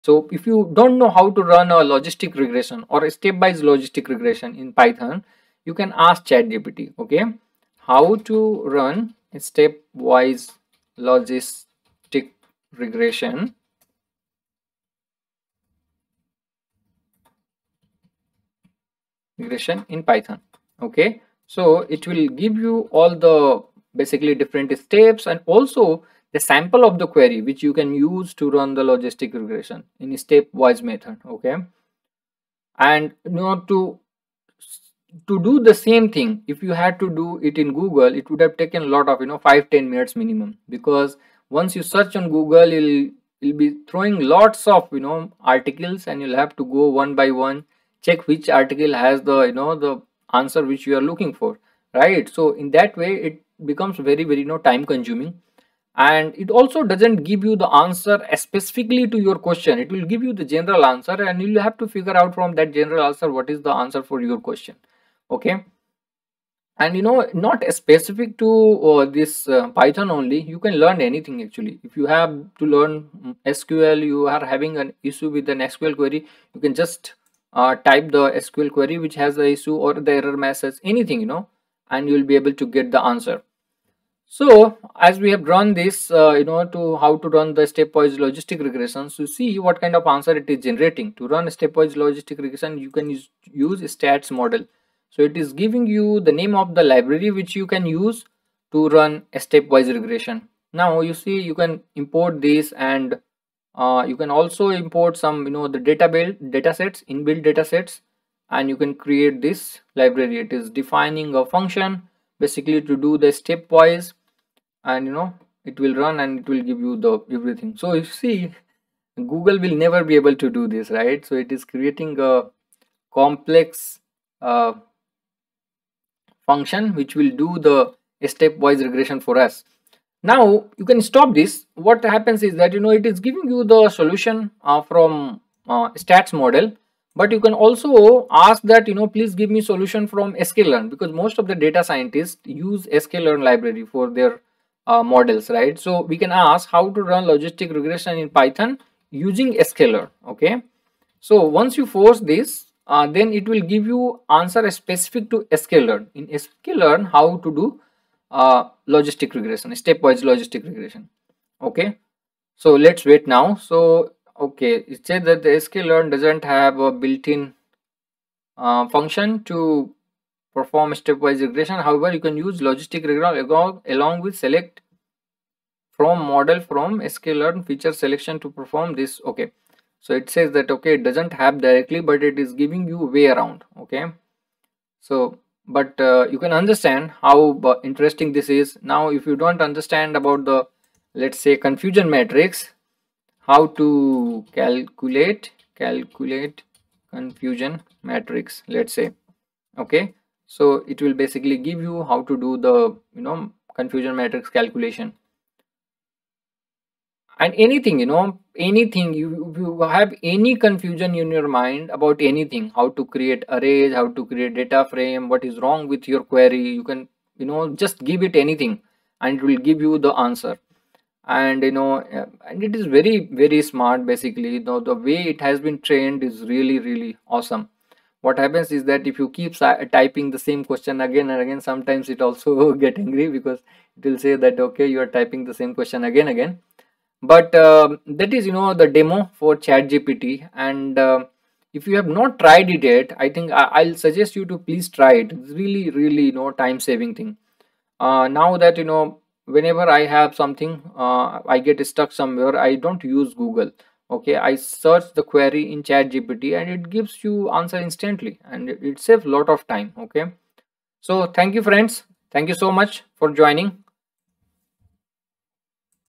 LOGISTIC REGRESSION USING CHATGPT
ChatGPT can be used to learn new things. You can use it to build data science models. Here we show you how you can use it to build a Logistic Regression model.